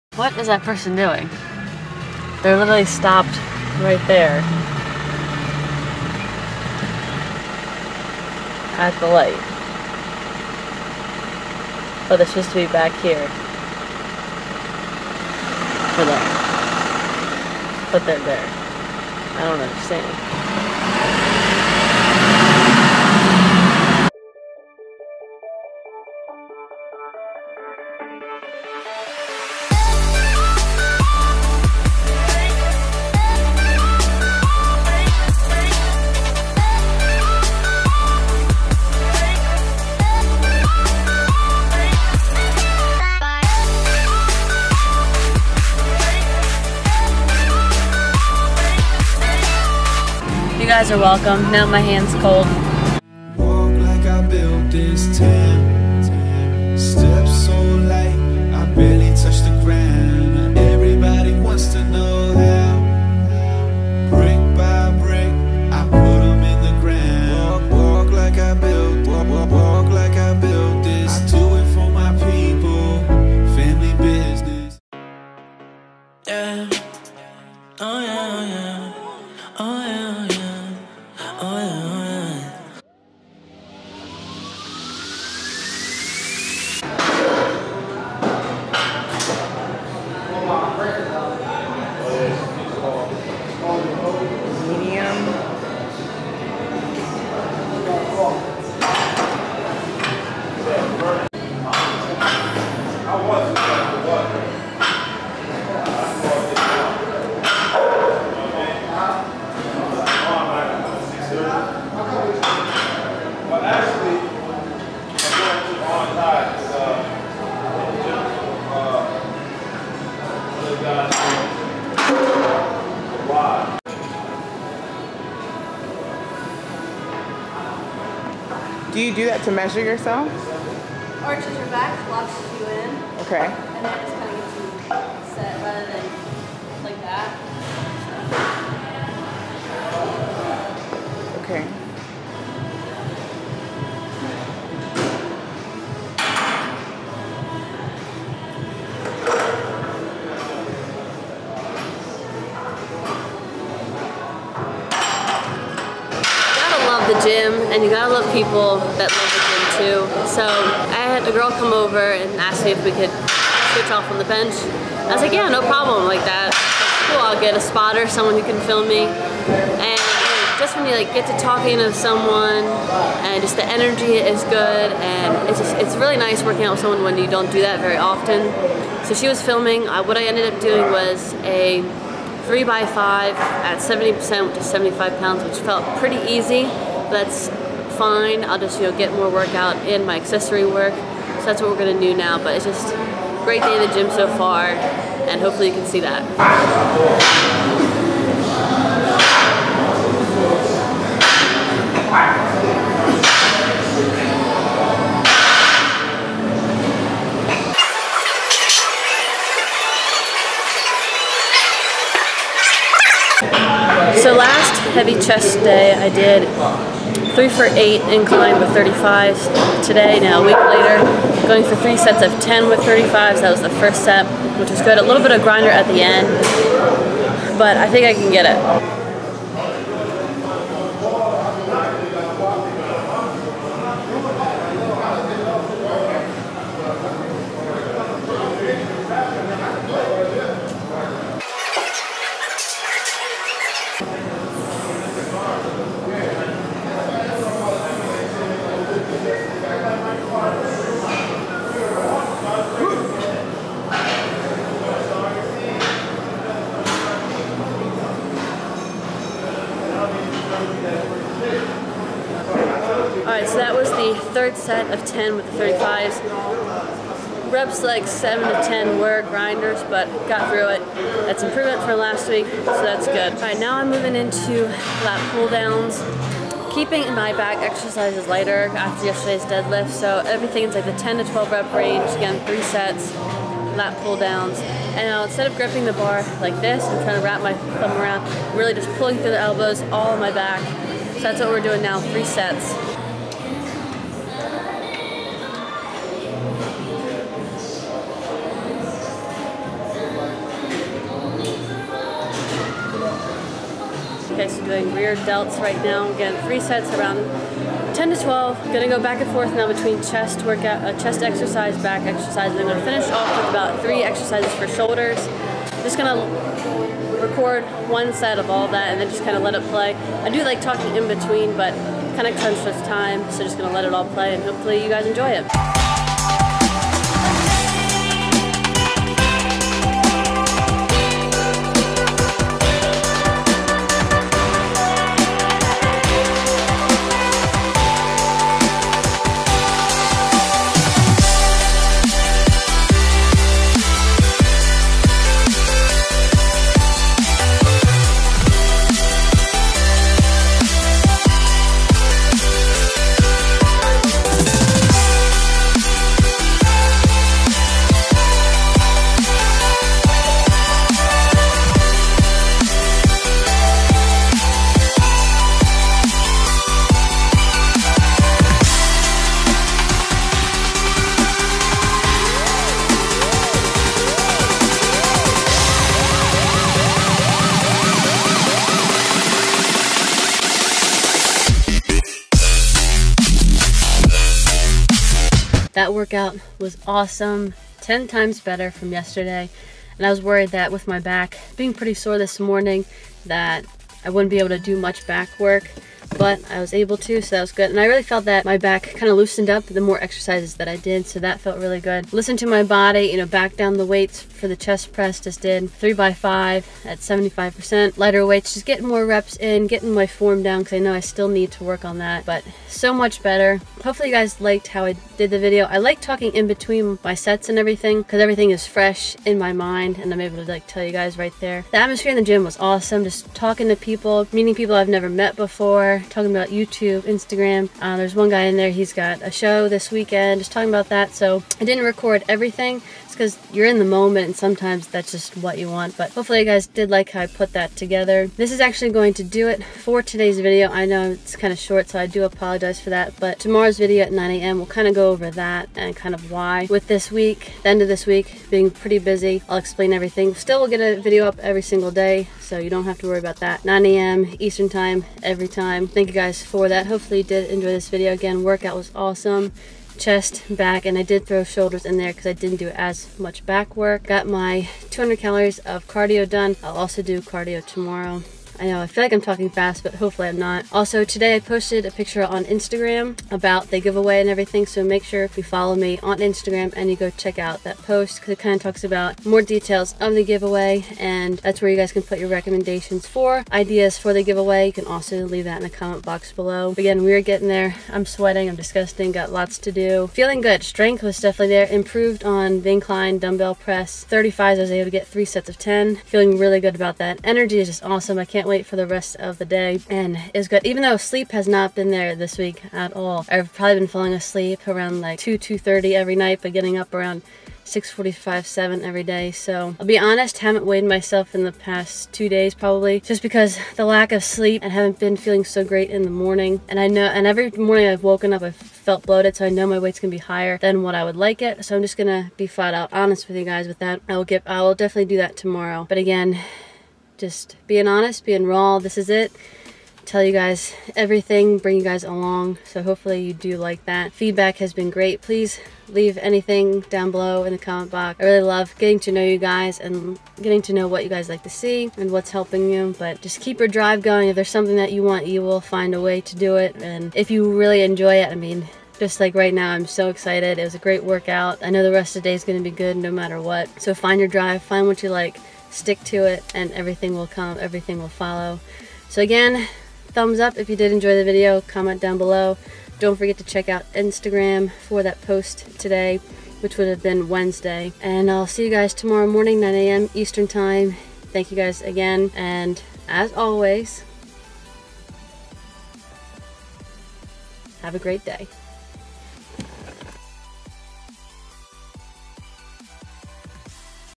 BP- Ep. 16: CHEST, SHOULDERS, BACK & GIVEAWAY SOON !
16th episode of the series! Progress is slowly but surely being made!
Thank you all for the support! Once we reach 500 on YouTube and 2,000 on Instagram there will be a giveaway from each! :)